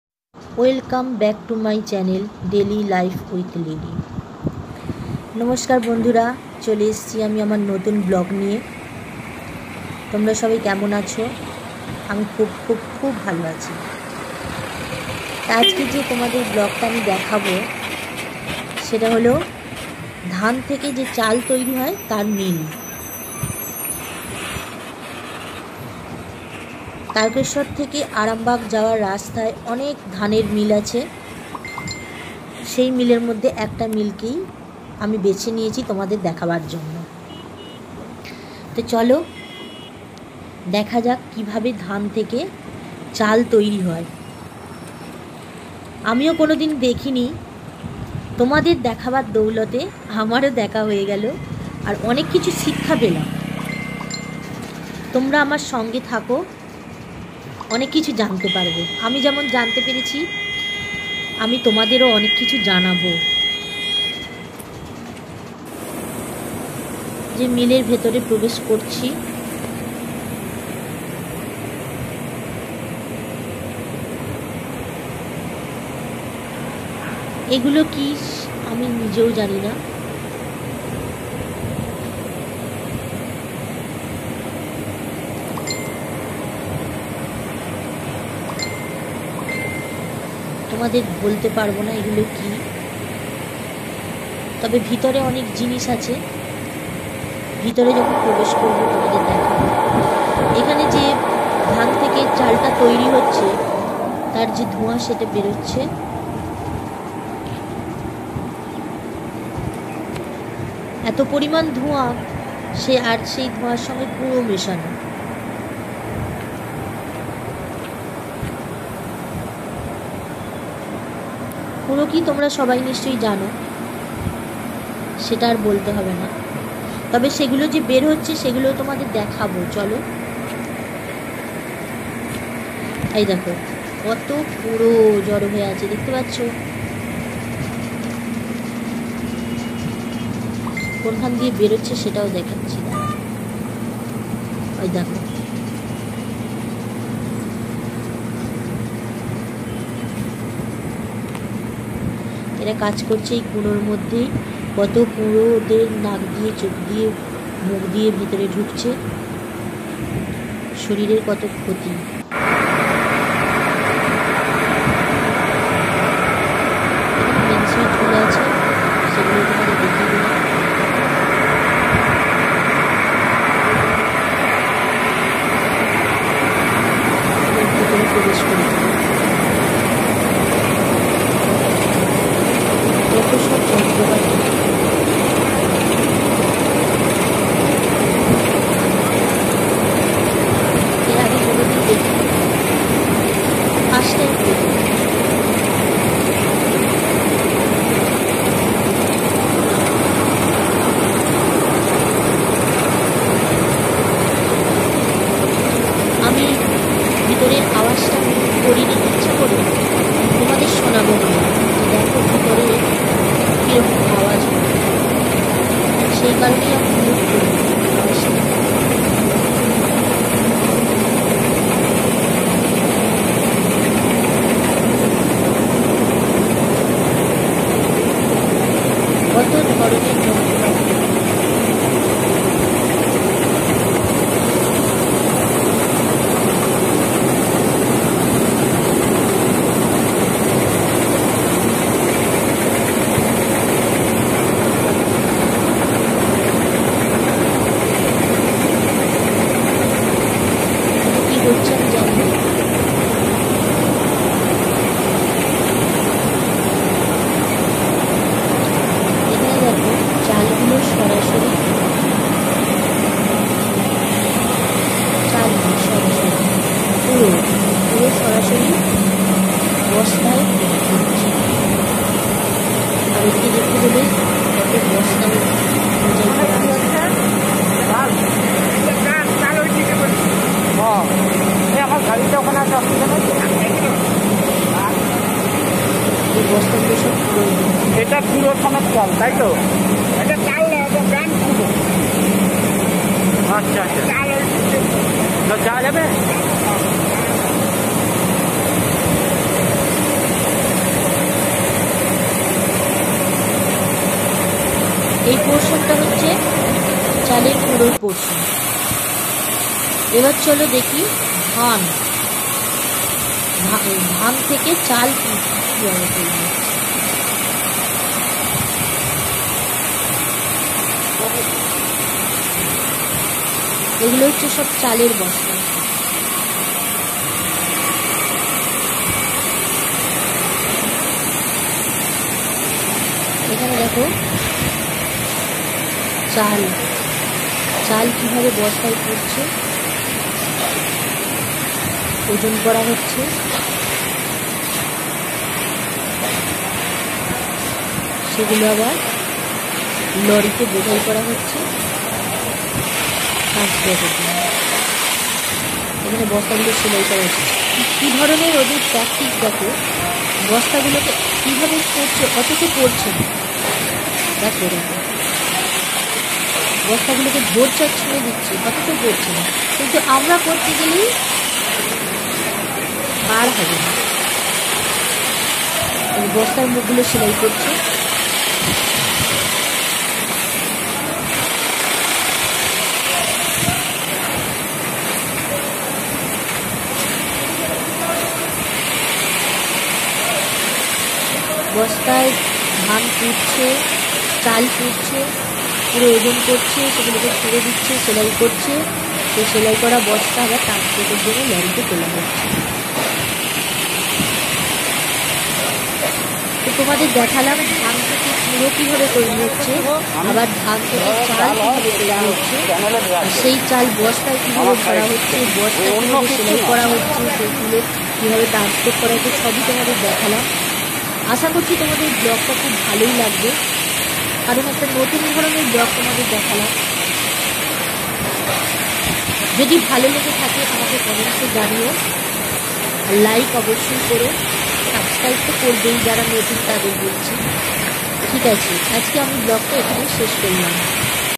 Automatic RICE MILL in Bengal// how it's made - paddy to rice
here we are showcasing the process of converting paddy to rice in one of the old and traditional ricemill functioning in west Bengal. hope you all like this video. please subscribe to my channel for more updates.